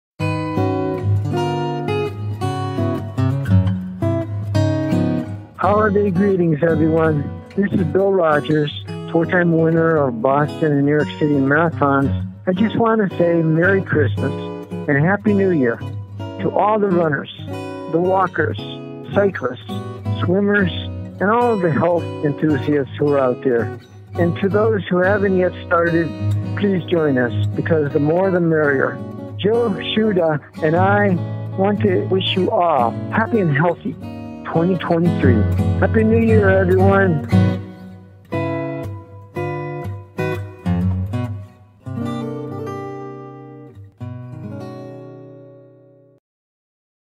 Holiday Greetings from Bill Rodgers
Holiday greetings from "Boston Billy" Rodgers, four-time winner of the Boston Marathon and the New York City Marathon.

Boston Marathon photos courtesy of Jeff Johnoson/RunMoreMiles.com
and the Boston Athletic Association /FayFoto
2016 New York Marathon Expo courtesy of NYRR.